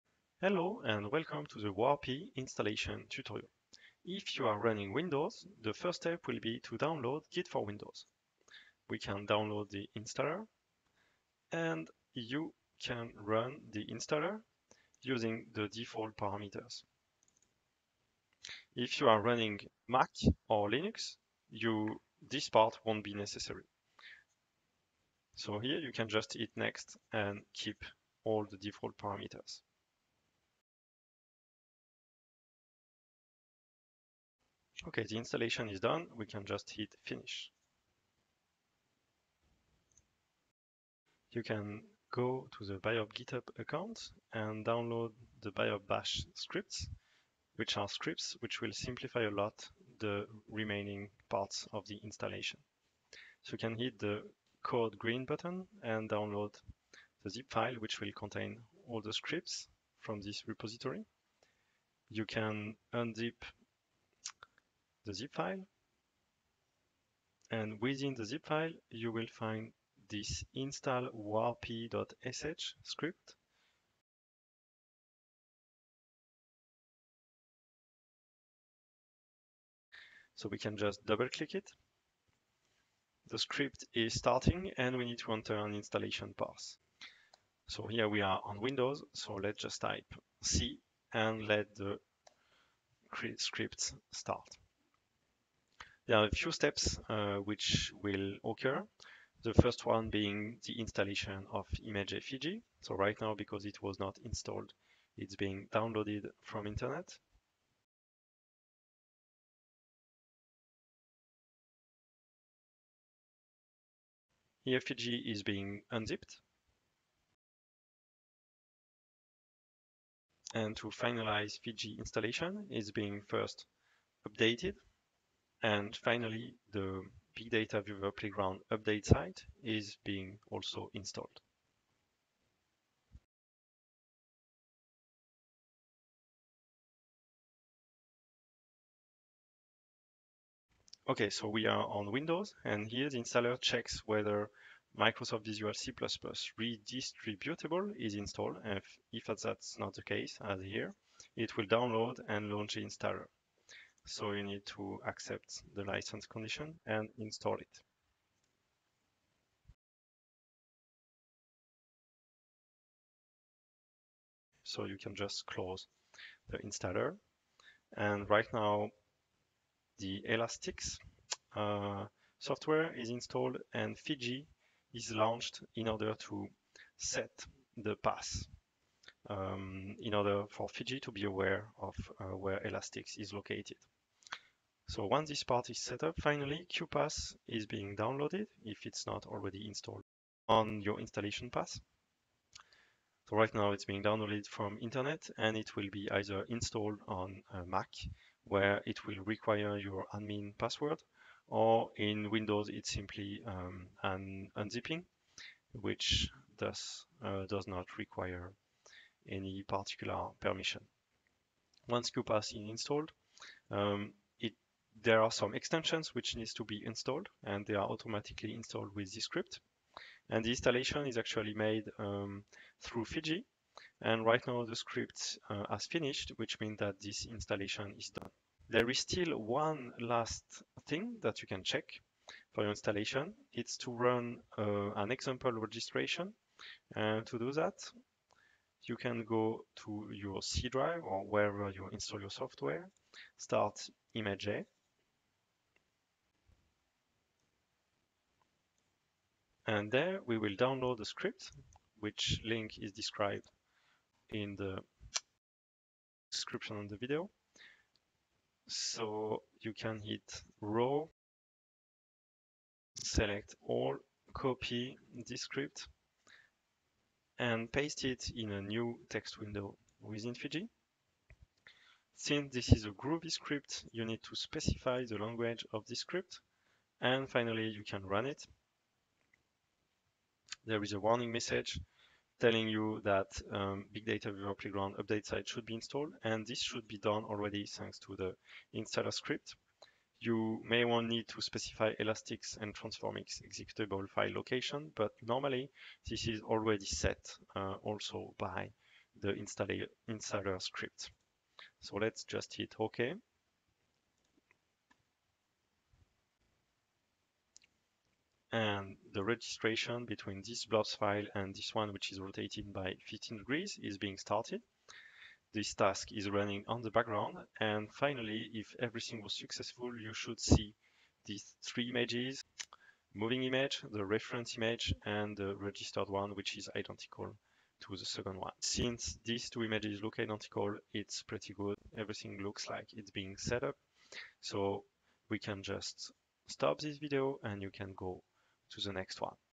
1 - Installation | Warpy: Whole Slide Images Registration with QuPath and Fiji
This tutorial presents how to install components for the wsi registration workflow documented

Linked videos:
1 - Installation : this video